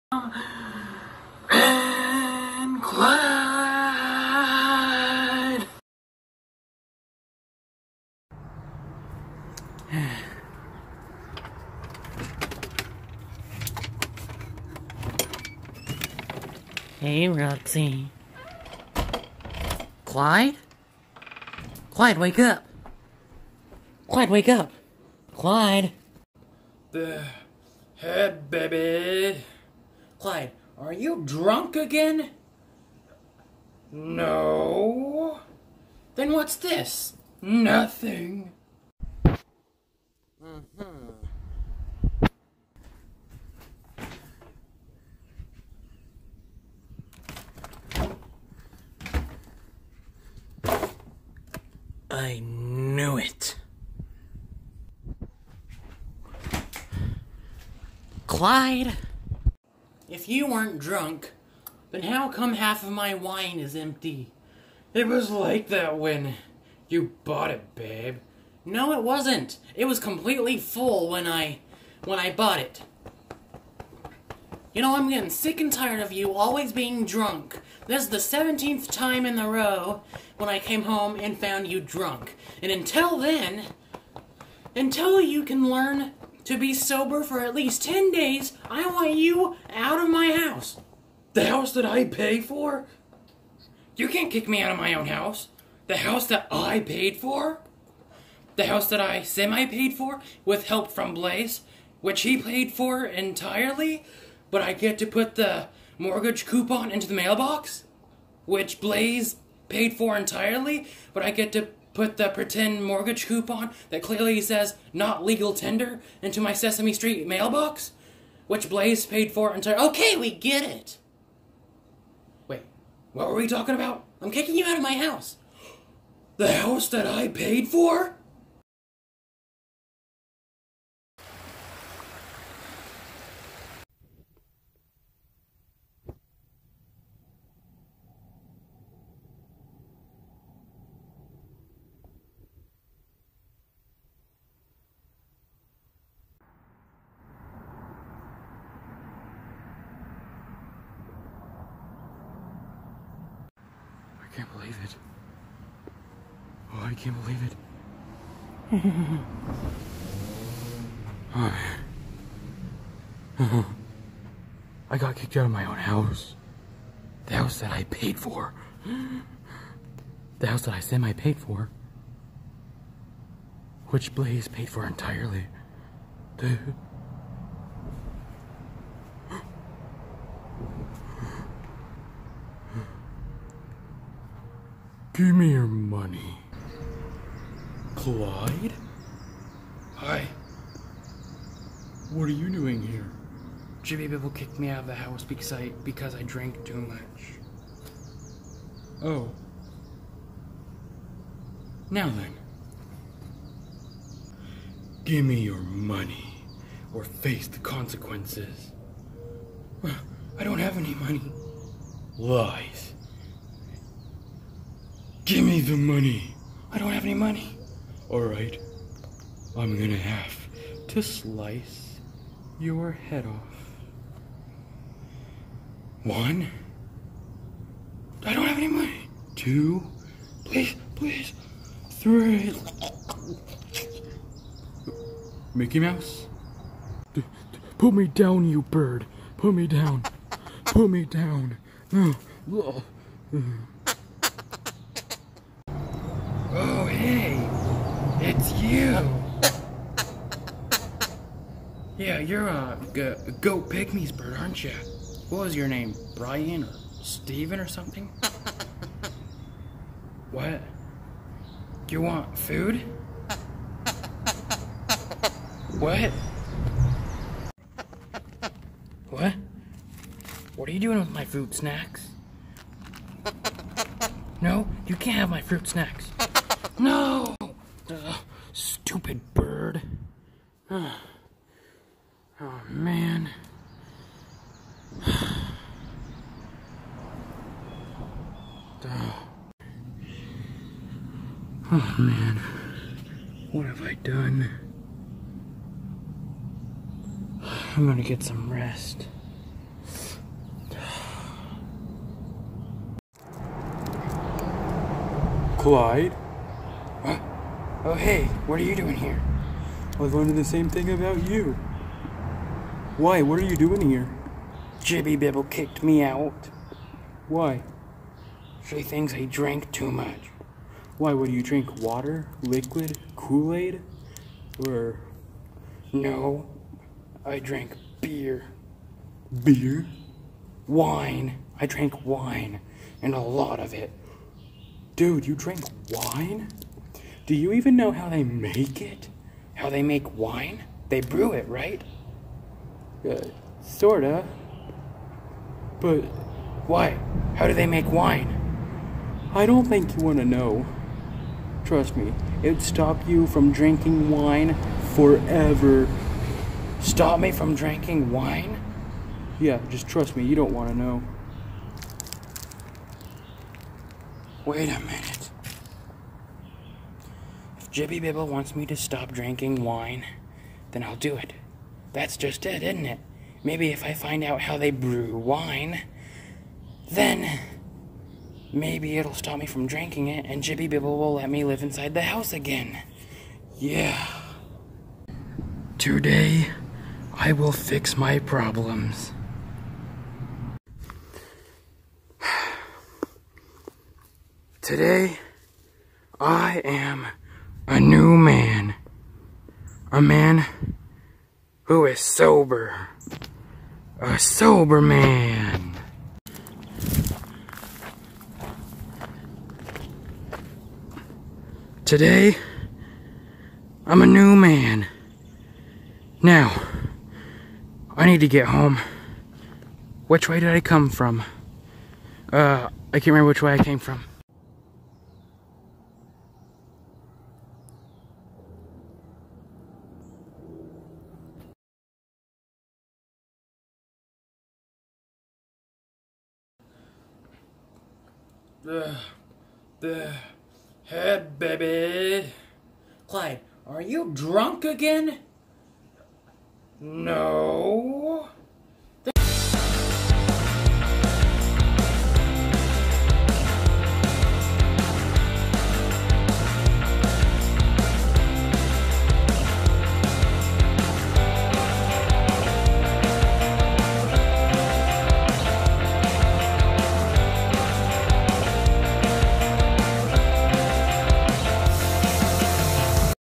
Clyde's Channel Short: Clyde's Bridge
Clyde gets kicked out of the house and is living under a bridge.